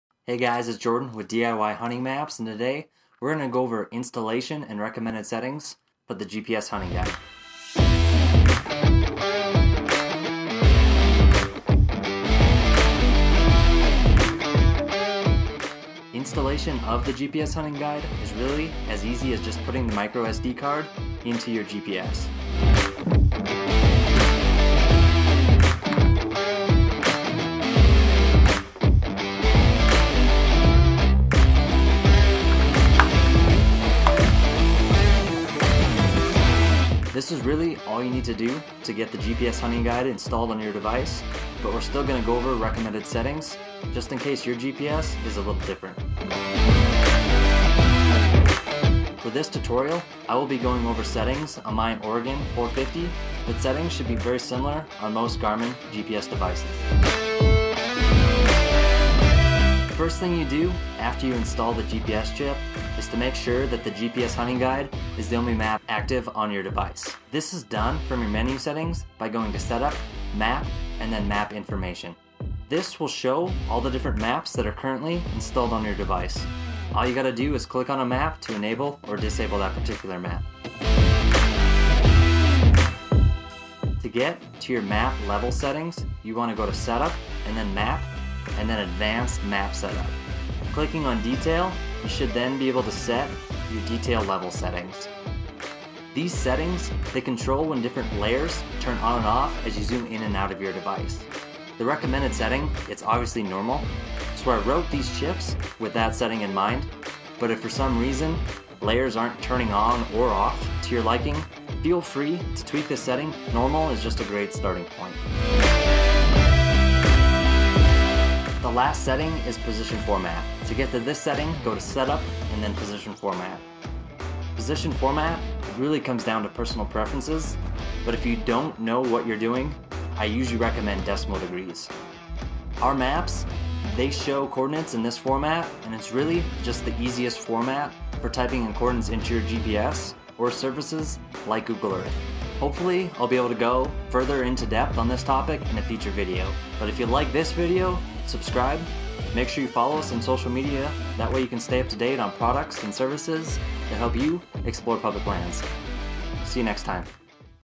GPS Chip Installation and Recommended Settings | DIY Hunting Maps
Learn how to install DIY Hunting Maps' 'The Digital Hunting Guide' in any Garmin GPS device.  At the same time, learn recommended settings for using and running custom maps on your GPS.  These settings include: Position Format, Map Detail, and Enabling Maps.